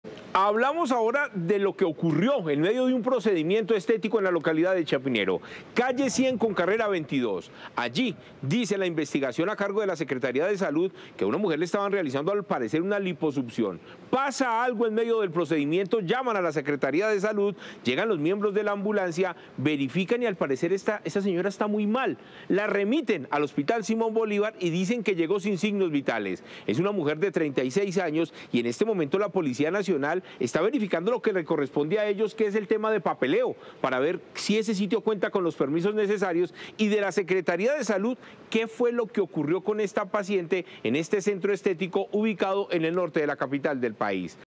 Investigan muerte de mujer tras una liposucción en centro estético de Bogotá
Las autoridades en Bogotá están investigando la muerte de una mujer, de 36 años, a la que le realizaron una liposucción en un centro estético ubicado en la localidad de Chapinero, norte de la ciudad.

Según información preliminar, en el momento del procedimiento sucede algo que prende las alarmas y llaman a la Secretaría de Salud. Una ambulancia llega al sitio, verifica el estado de la mujer, quien al parecer se encontraba muy mal.

Debido a esto la paciente fue remitida al Hospital Simón Bolívar, donde llegó sin signos vitales.